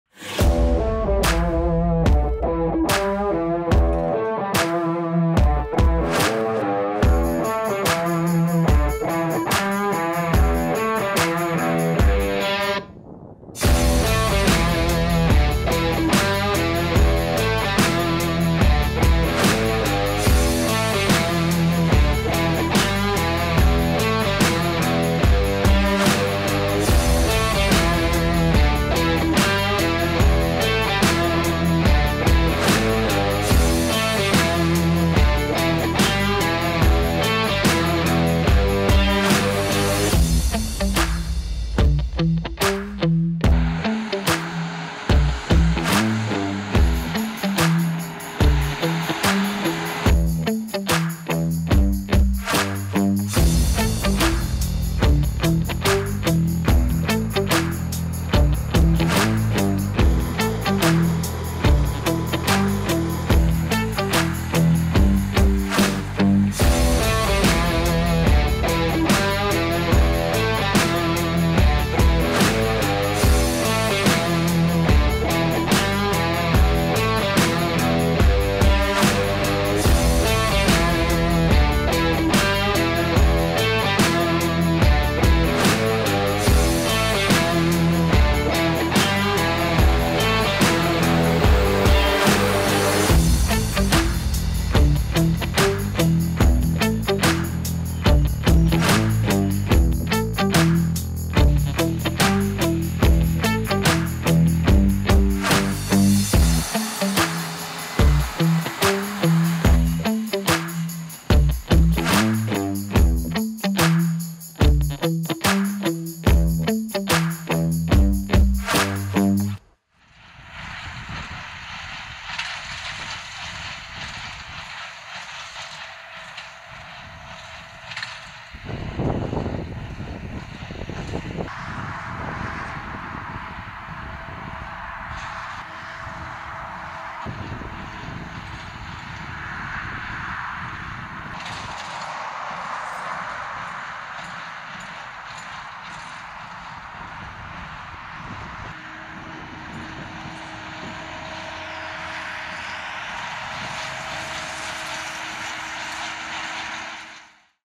High speed baling with New Holland | Straw harvest 2024 | Top secret testing?
Cereal harvest 2024 has started in the Netherlands with the winter barley. That means also the start of straw harvest. By accident we stumble upon a combination from New Holland themselves. This is a test rig from the factory in Zedelgem which is used to test the durability or wearing of the parts for example. It is certainly flying along in the field!

On the roof of the New Holland tractor you see a Lidar sensor. This is used to detect the swaths of straw (or grass) and the tractor will steer itself automatically. IntelliSense is what New Holland calls it and this was launched last Agritechnica.

In the same field we also find the other end of the spectrum. A Case IH Maxxum 115 is making small square bales (also known as idiot blocks ;-)). with a Massey Ferguson baler and Meijer sledge.